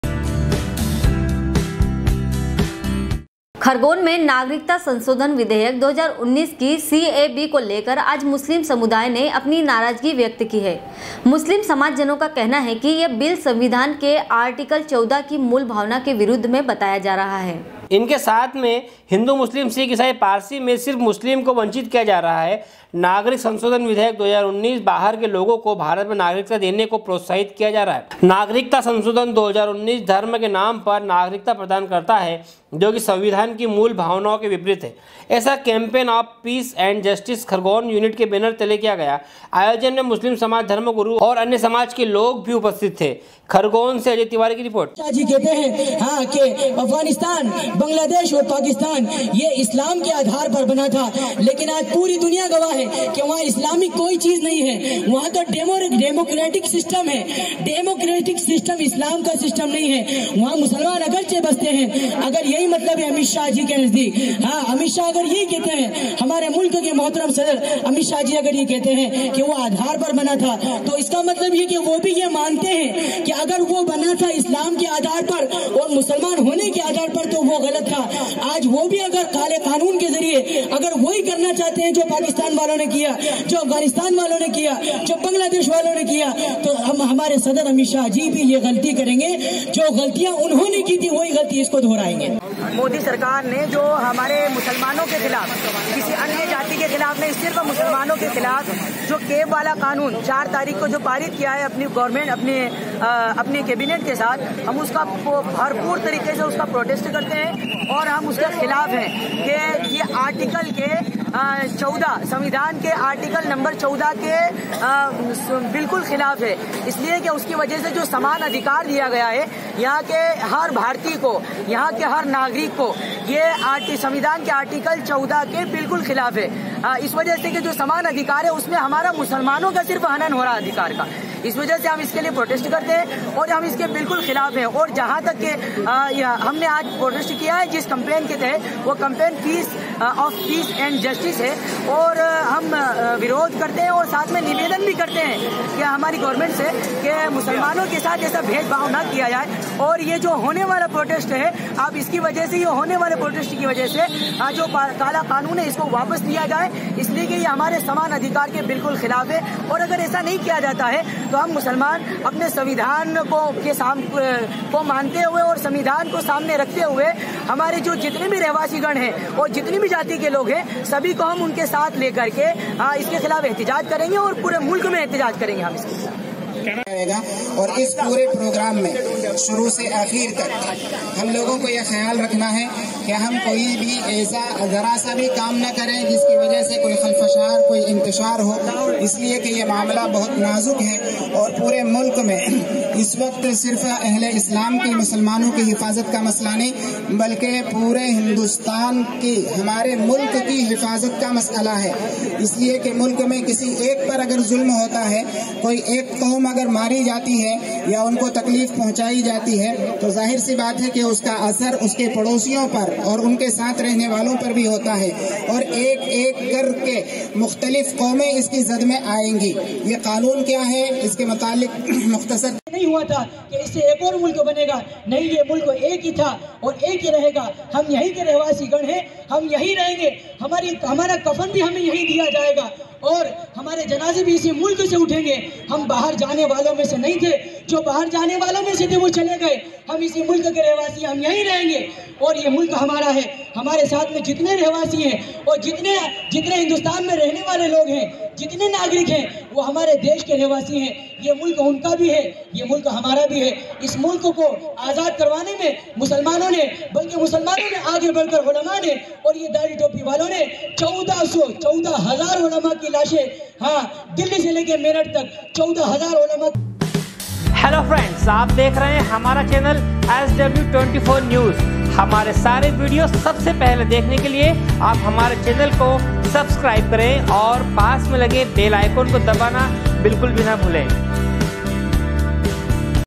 sw24news khargone, 14.11.2019
खरगोन में नागरिकता संशोधन विधेयक 2019 की सीएबी को लेकर आज मुस्लिम समुदाय ने अपनी नाराजगी व्यक्त की है। मुस्लिम समाज जनों का कहना है कि यह बिल संविधान के आर्टिकल 14 की मूल भावना के विरोध में बताया जा रहा है खरगोन से अजय तिवारी की रिपेार्ट